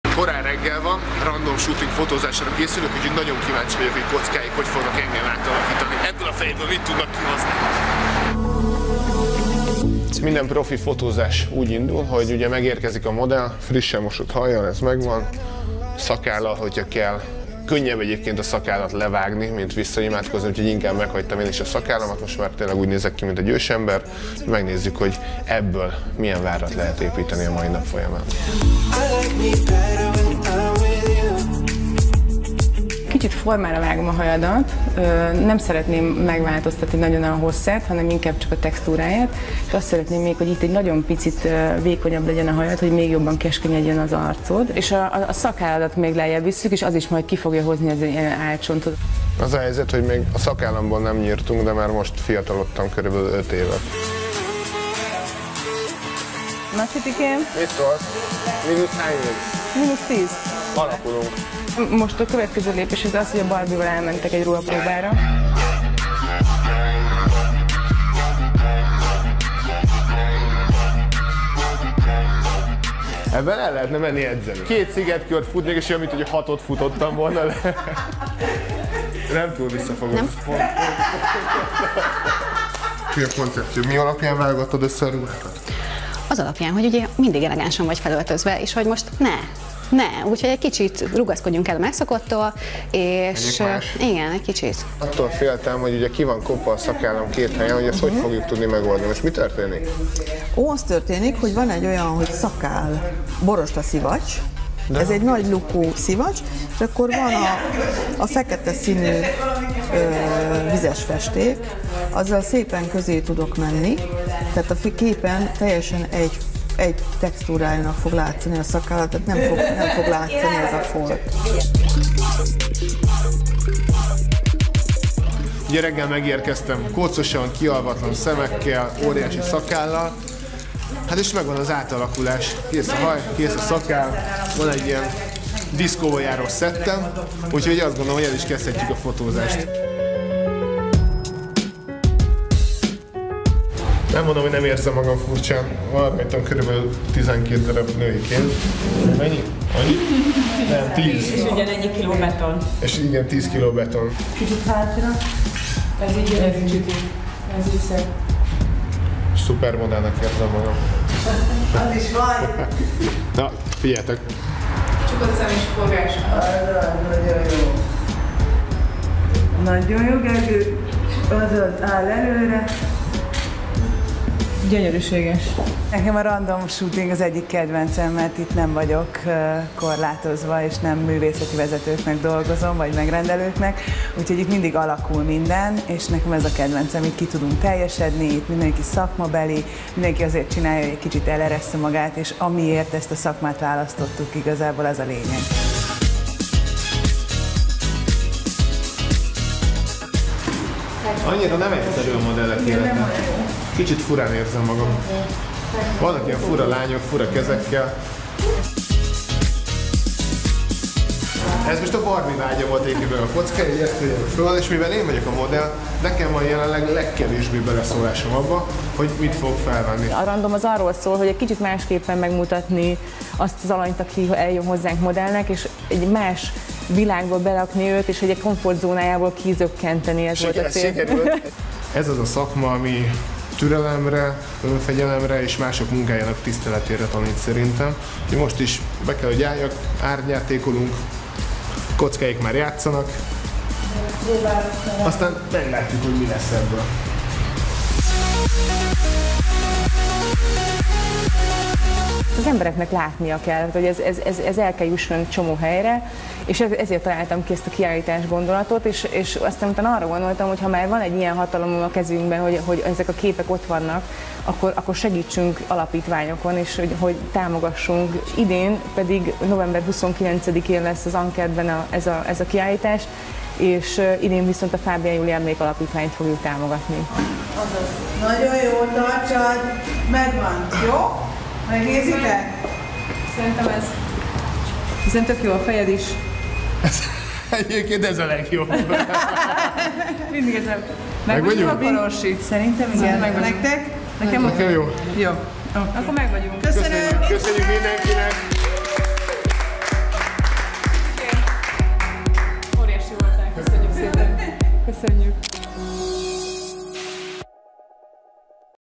Random Shooting – Csiti Koczkáék markában
Korán reggeli fej, frissen mosott haj – ezt hozta Csiti. Fiatalító hajnyírás Koczkától, extravagáns ruhák Rácz Barbitól és borostaszivacs a sminkben Dévényi Kathytól. Ezt kapta a Random Shootig Day stábjától. Meg mínusz tíz évet. És 11 női kezet meg 10 kg betont... nézd meg Hajas Orsolya képein! Te bevállalnád?